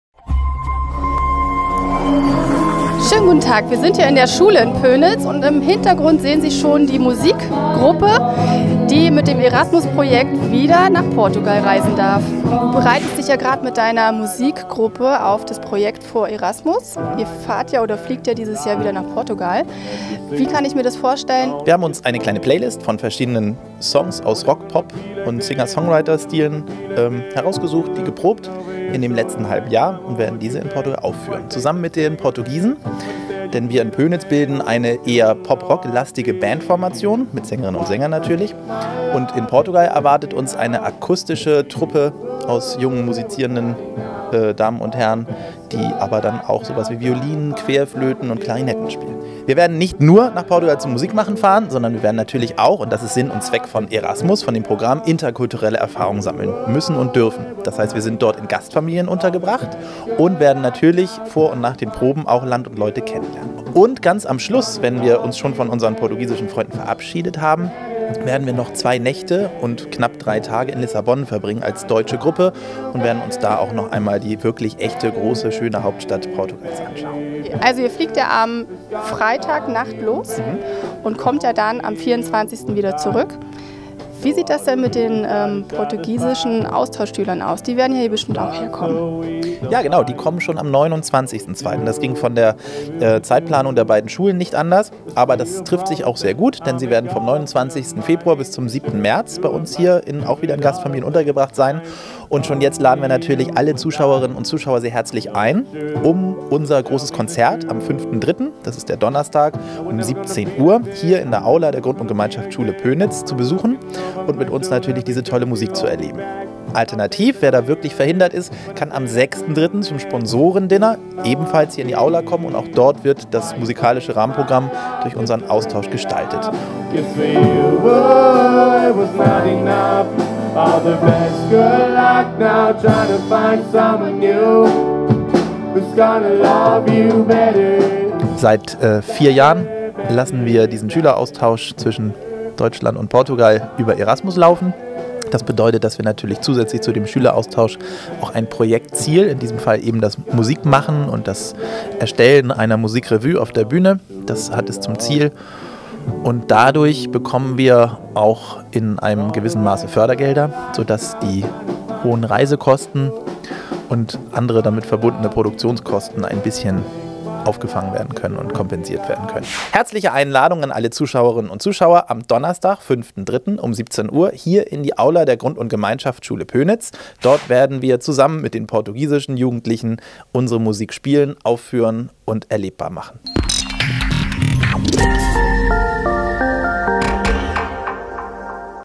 Erasmus 2020: Schüleraustausch Pönitz – Lagoa (Portugal)
Die Portugal-AG der Schule Pönitz fliegt am Freitag mit vielen Musikstücken im Gepäck zum Schüleraustausch in die Algarve.
Heute wurde nochmals ordentlich geprobt. Wir durften im Publikum sitzen ...